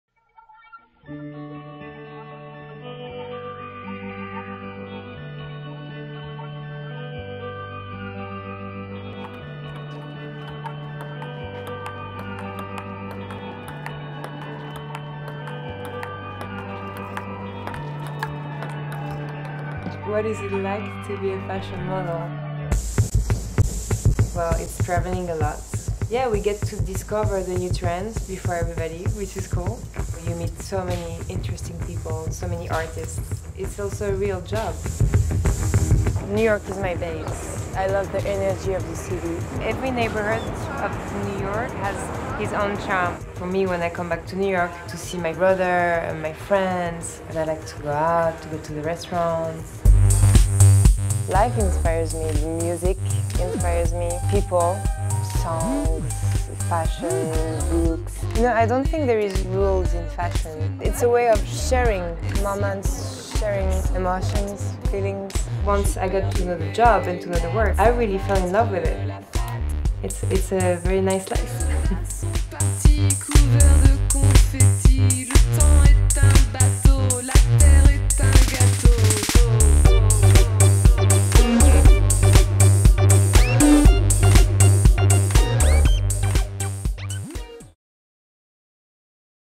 Constance Jablonski: A Day in the Life of an Estée Lauder Model
Take a walk with Estée Lauder spokesmodel Constance Jablonski as she explores New York City, talks about working and playing, and shares her favorite spots.